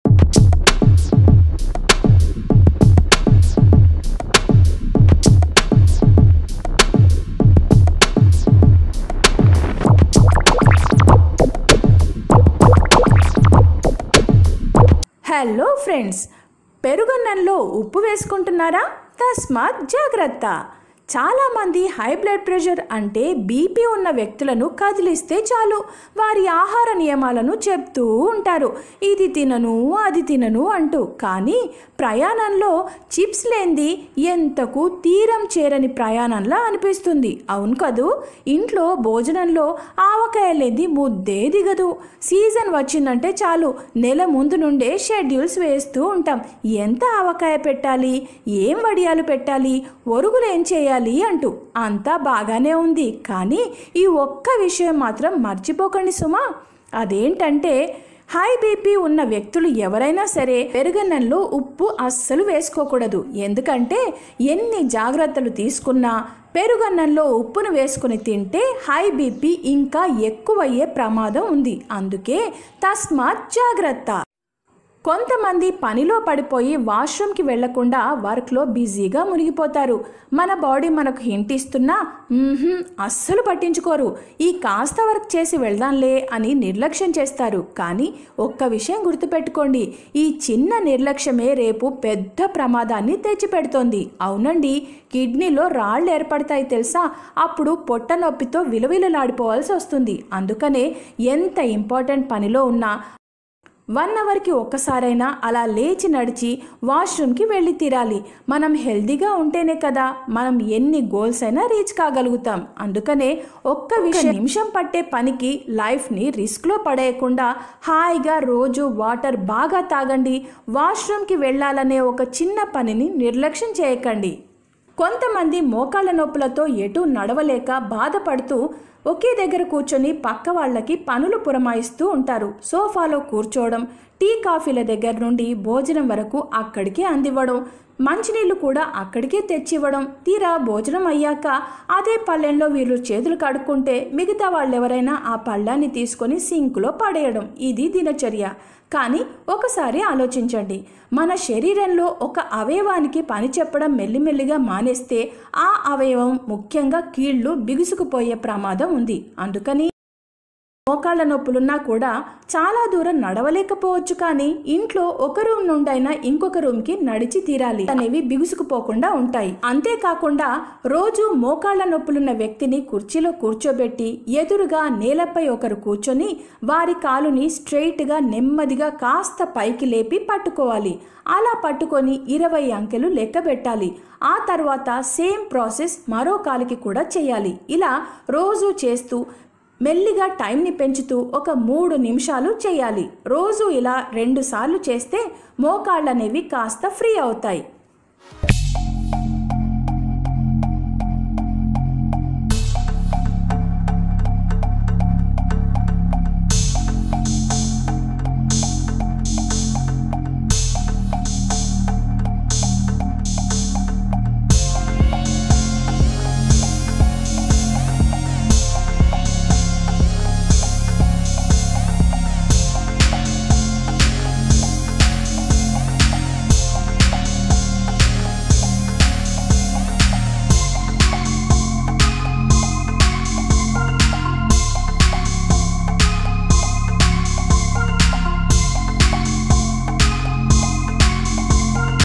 Health tips for Blood pressure and joint pains
Published by Chandra Varshini YouTube channel
This video explains the health tips which should be followed by people who are suffering with blood pressure and joint pains...